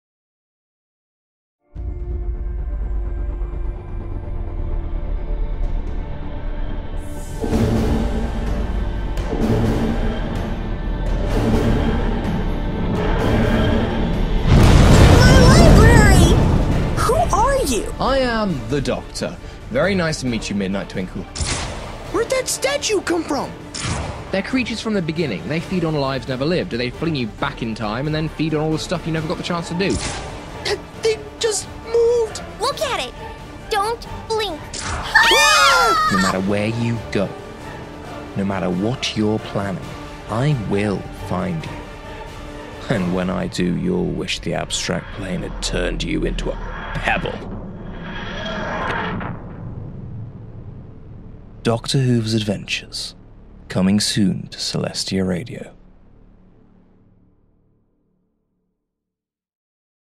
Doctor Whooves Adventures: Coming Soon
*NOTE: READ ME SERIOUSLY*

NEXT ONE SATURDAY FEBURARY 25TH AT 6PM EST

That is all. Listen then. May you ever be wibbly wobbly.

Exciting ain't it? A trailer for the upcoming plays, which will be airing on Celestia radio very soon. The exact date isn't set in stone, but for dates and times be sure to check out the blog at : Ponyinaboxproductions.wordpress.com and be sure to listen to Celestia Radio at ponify.me

They will also appear on this YT channel 24 Hours after airing.

Trailer was put together by Dash, who we all owe a large hug,

Vectors by:
cheesefaceman1
NecronomiconOfGod
Railrugs
isterLolrus
hombre0
nickman983
ashweez
FluffyWuffs
CptOfTheFriendship

The Doc was played by Jamjar
Twilight by Lauren Goodnight
Spike by Pony1kenobi
And Pinkie Pie by Flootershai